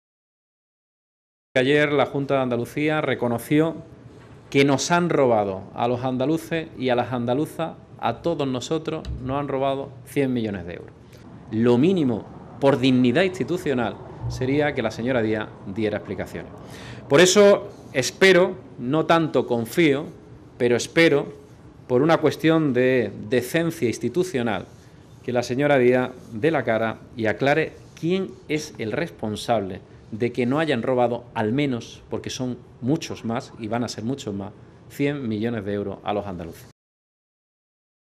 Juanma Moreno: "¿Quién es el responsable de que la Junta nos haya robado 100 millones de euros?”
Discurso del presidente del PP-A, Juanma Moreno, durante el encuentro con asociaciones de mujeres celebrado el día 5 de septiembre de 2014 en el restaurante Nantara Puerto de Sevilla.